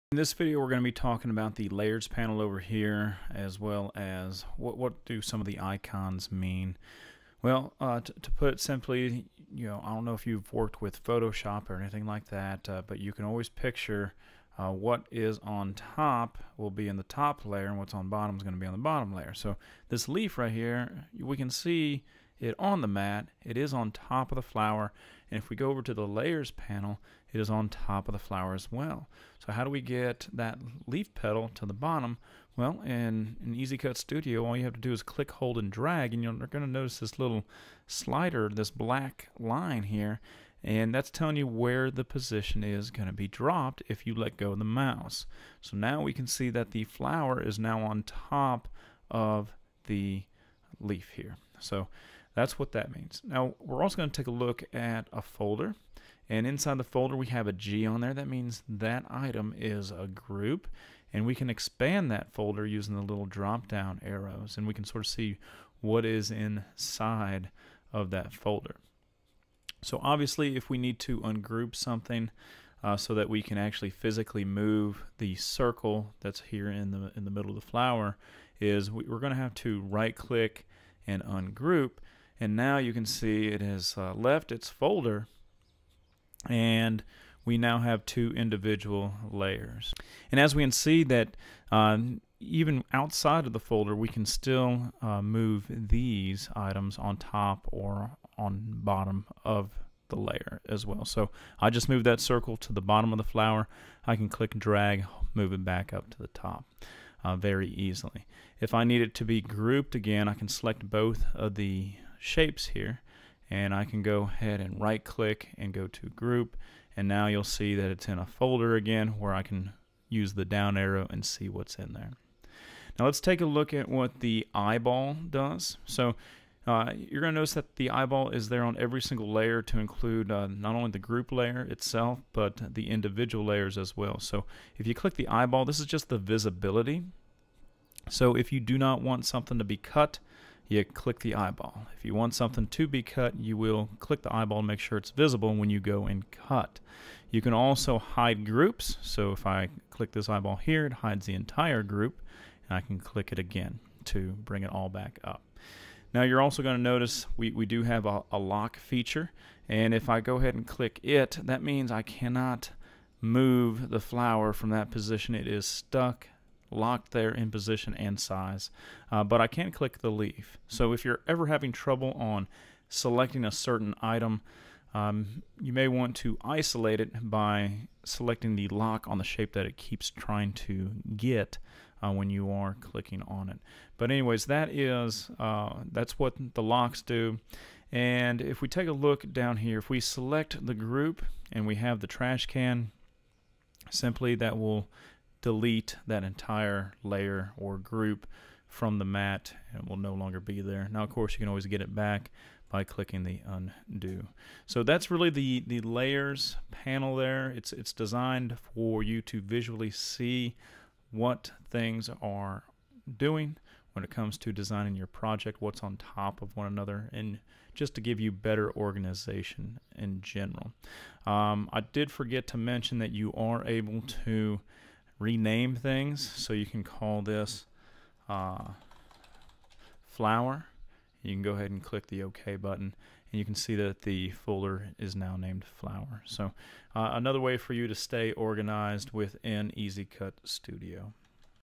Easy Cut Studio The Layer Menu
We demonstrate the importance of the layer menu. We show you how to group, hide, lock your layers in Easy Cut Studio.